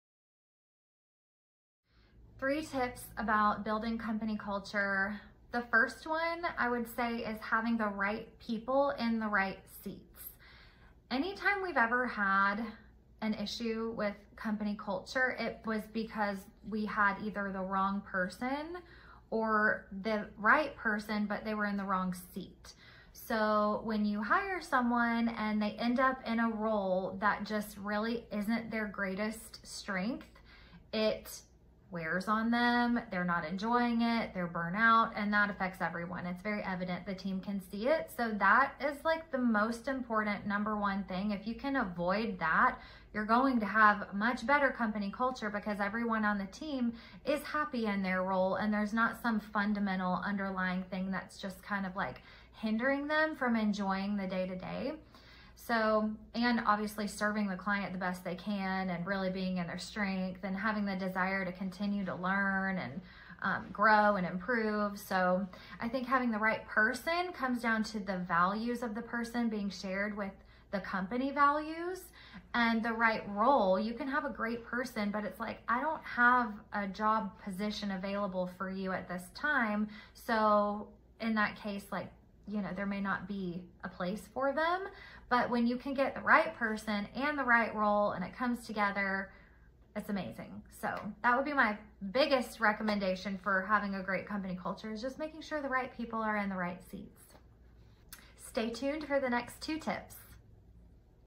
Ready to build a thriving company culture?
Our CEO, Alex, is sharing her top 3 secrets, starting with #1 ➡️ Placing the right people in the right roles

This ensures everyone can contribute their strengths and build a strong foundation together.

P.S. Want a deep dive into company culture cultivation? Grab yours using the link in our bio 🔗